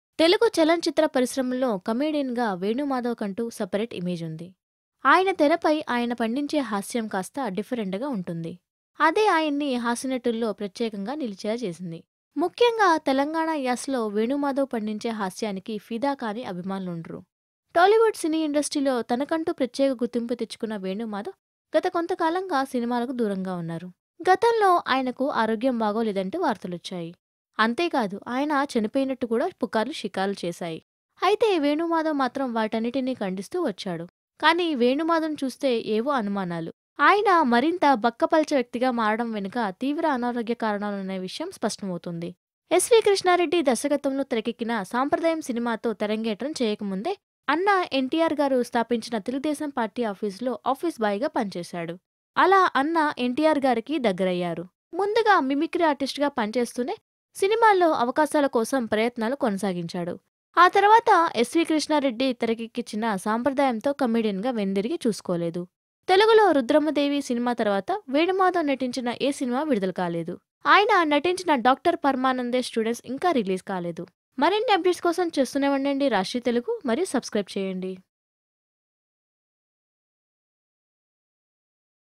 Telugu Comedian Venu Madhav Real Life story | Unknown Facts About Venu Madhav
Telugu Comedian Venu Madhav Real Life story, watch it only On Rajshri Telugu.